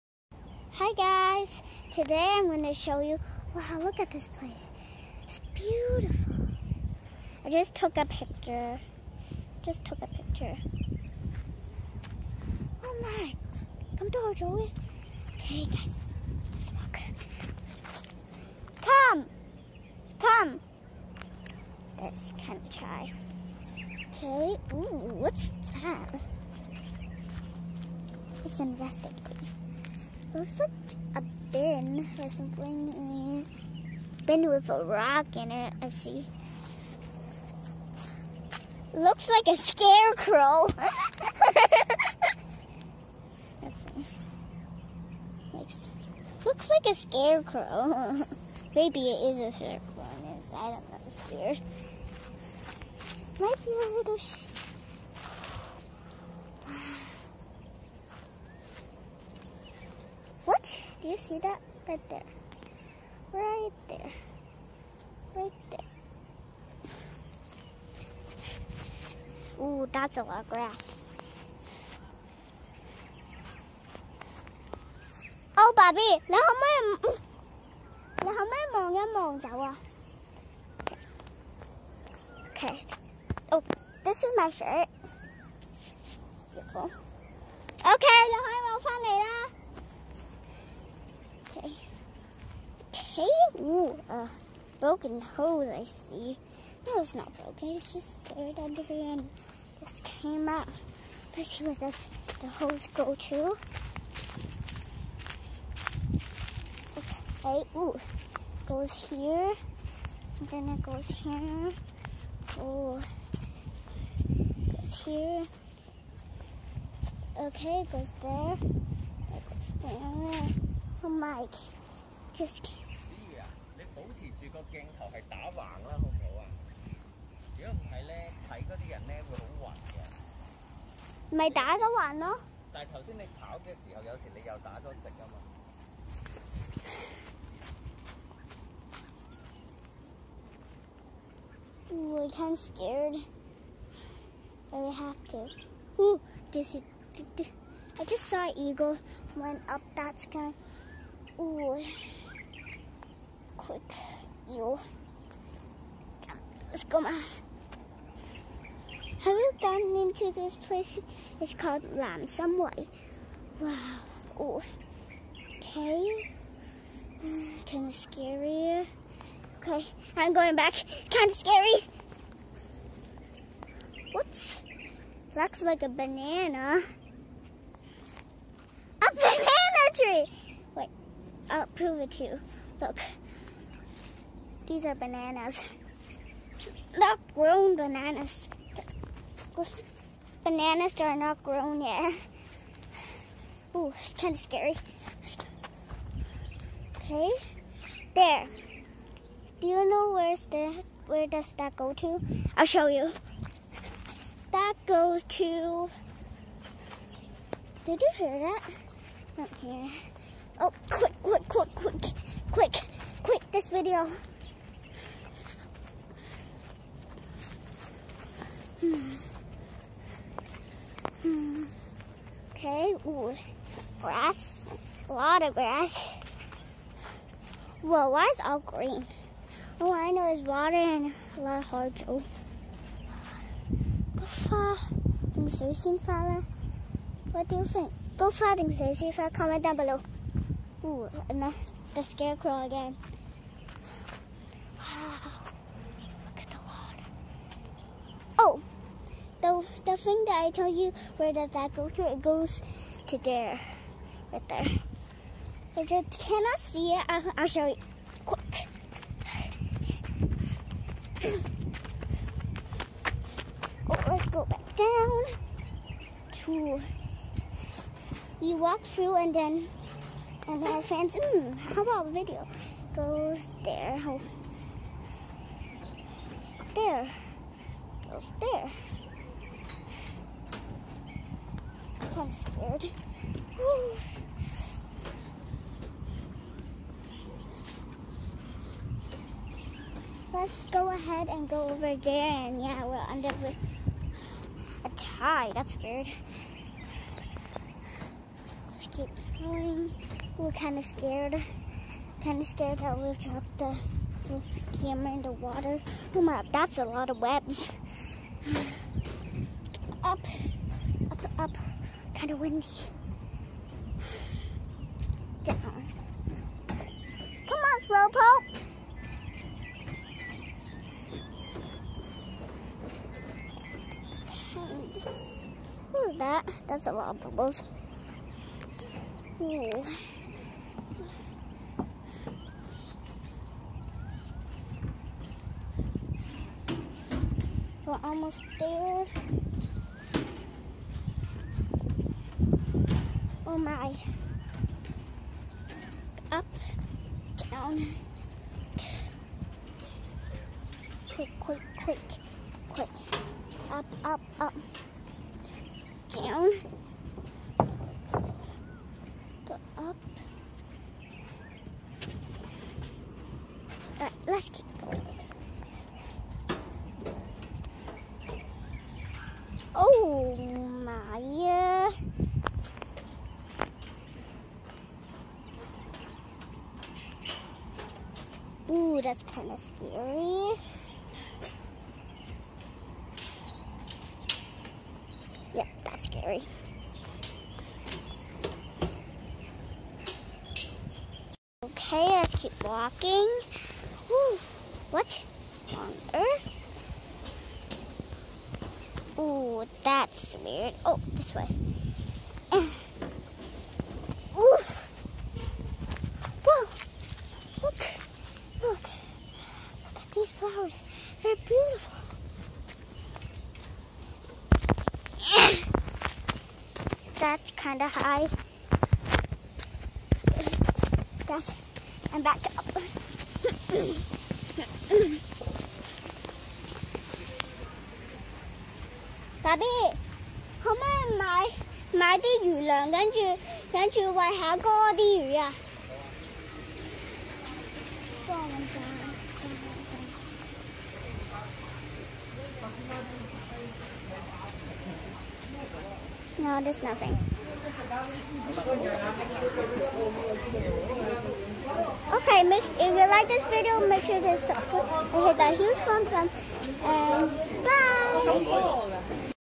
A Sunday adventure in Nam Sang Wai
there is a lake in one of the farm in Nam Sang Wai. see what Ashley explored  around the lake in this visit?